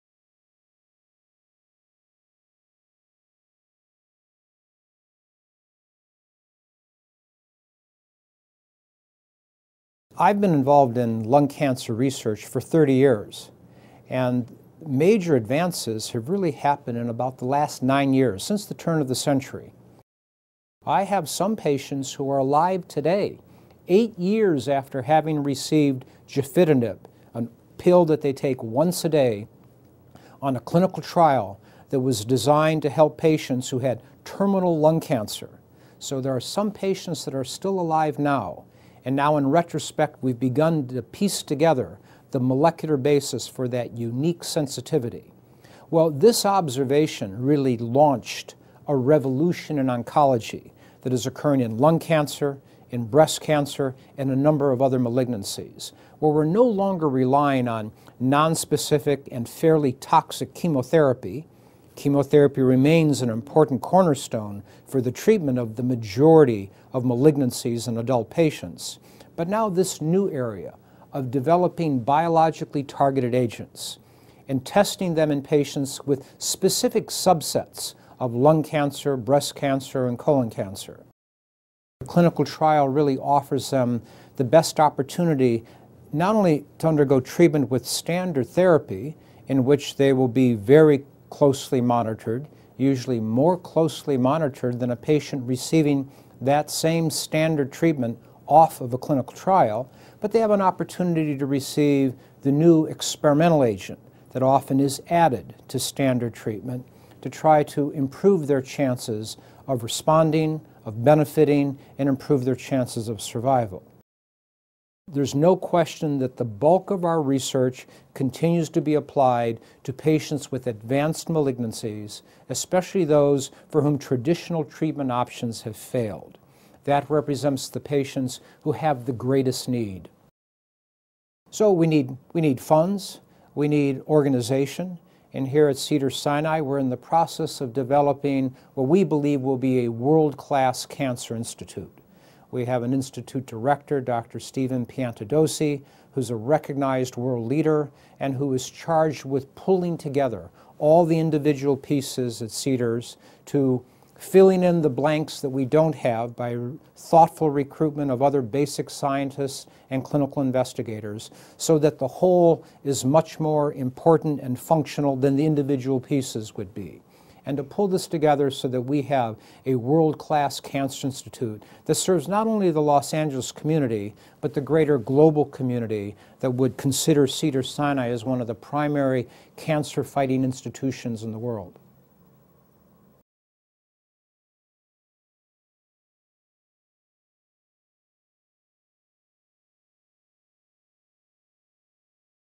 Innovations in Lung Cancer Research
Ronald Natale, MD, medical director of the Clinical Cancer Program at the Women's Guild Lung Institute, discusses innovations in lung cancer research. Dr. Natale is a nationally renowned oncologist with offices in the Samuel Oschin Cancer Center.

Dr. Natale was also featured in the Summer 2011 issue of Discoveries magazine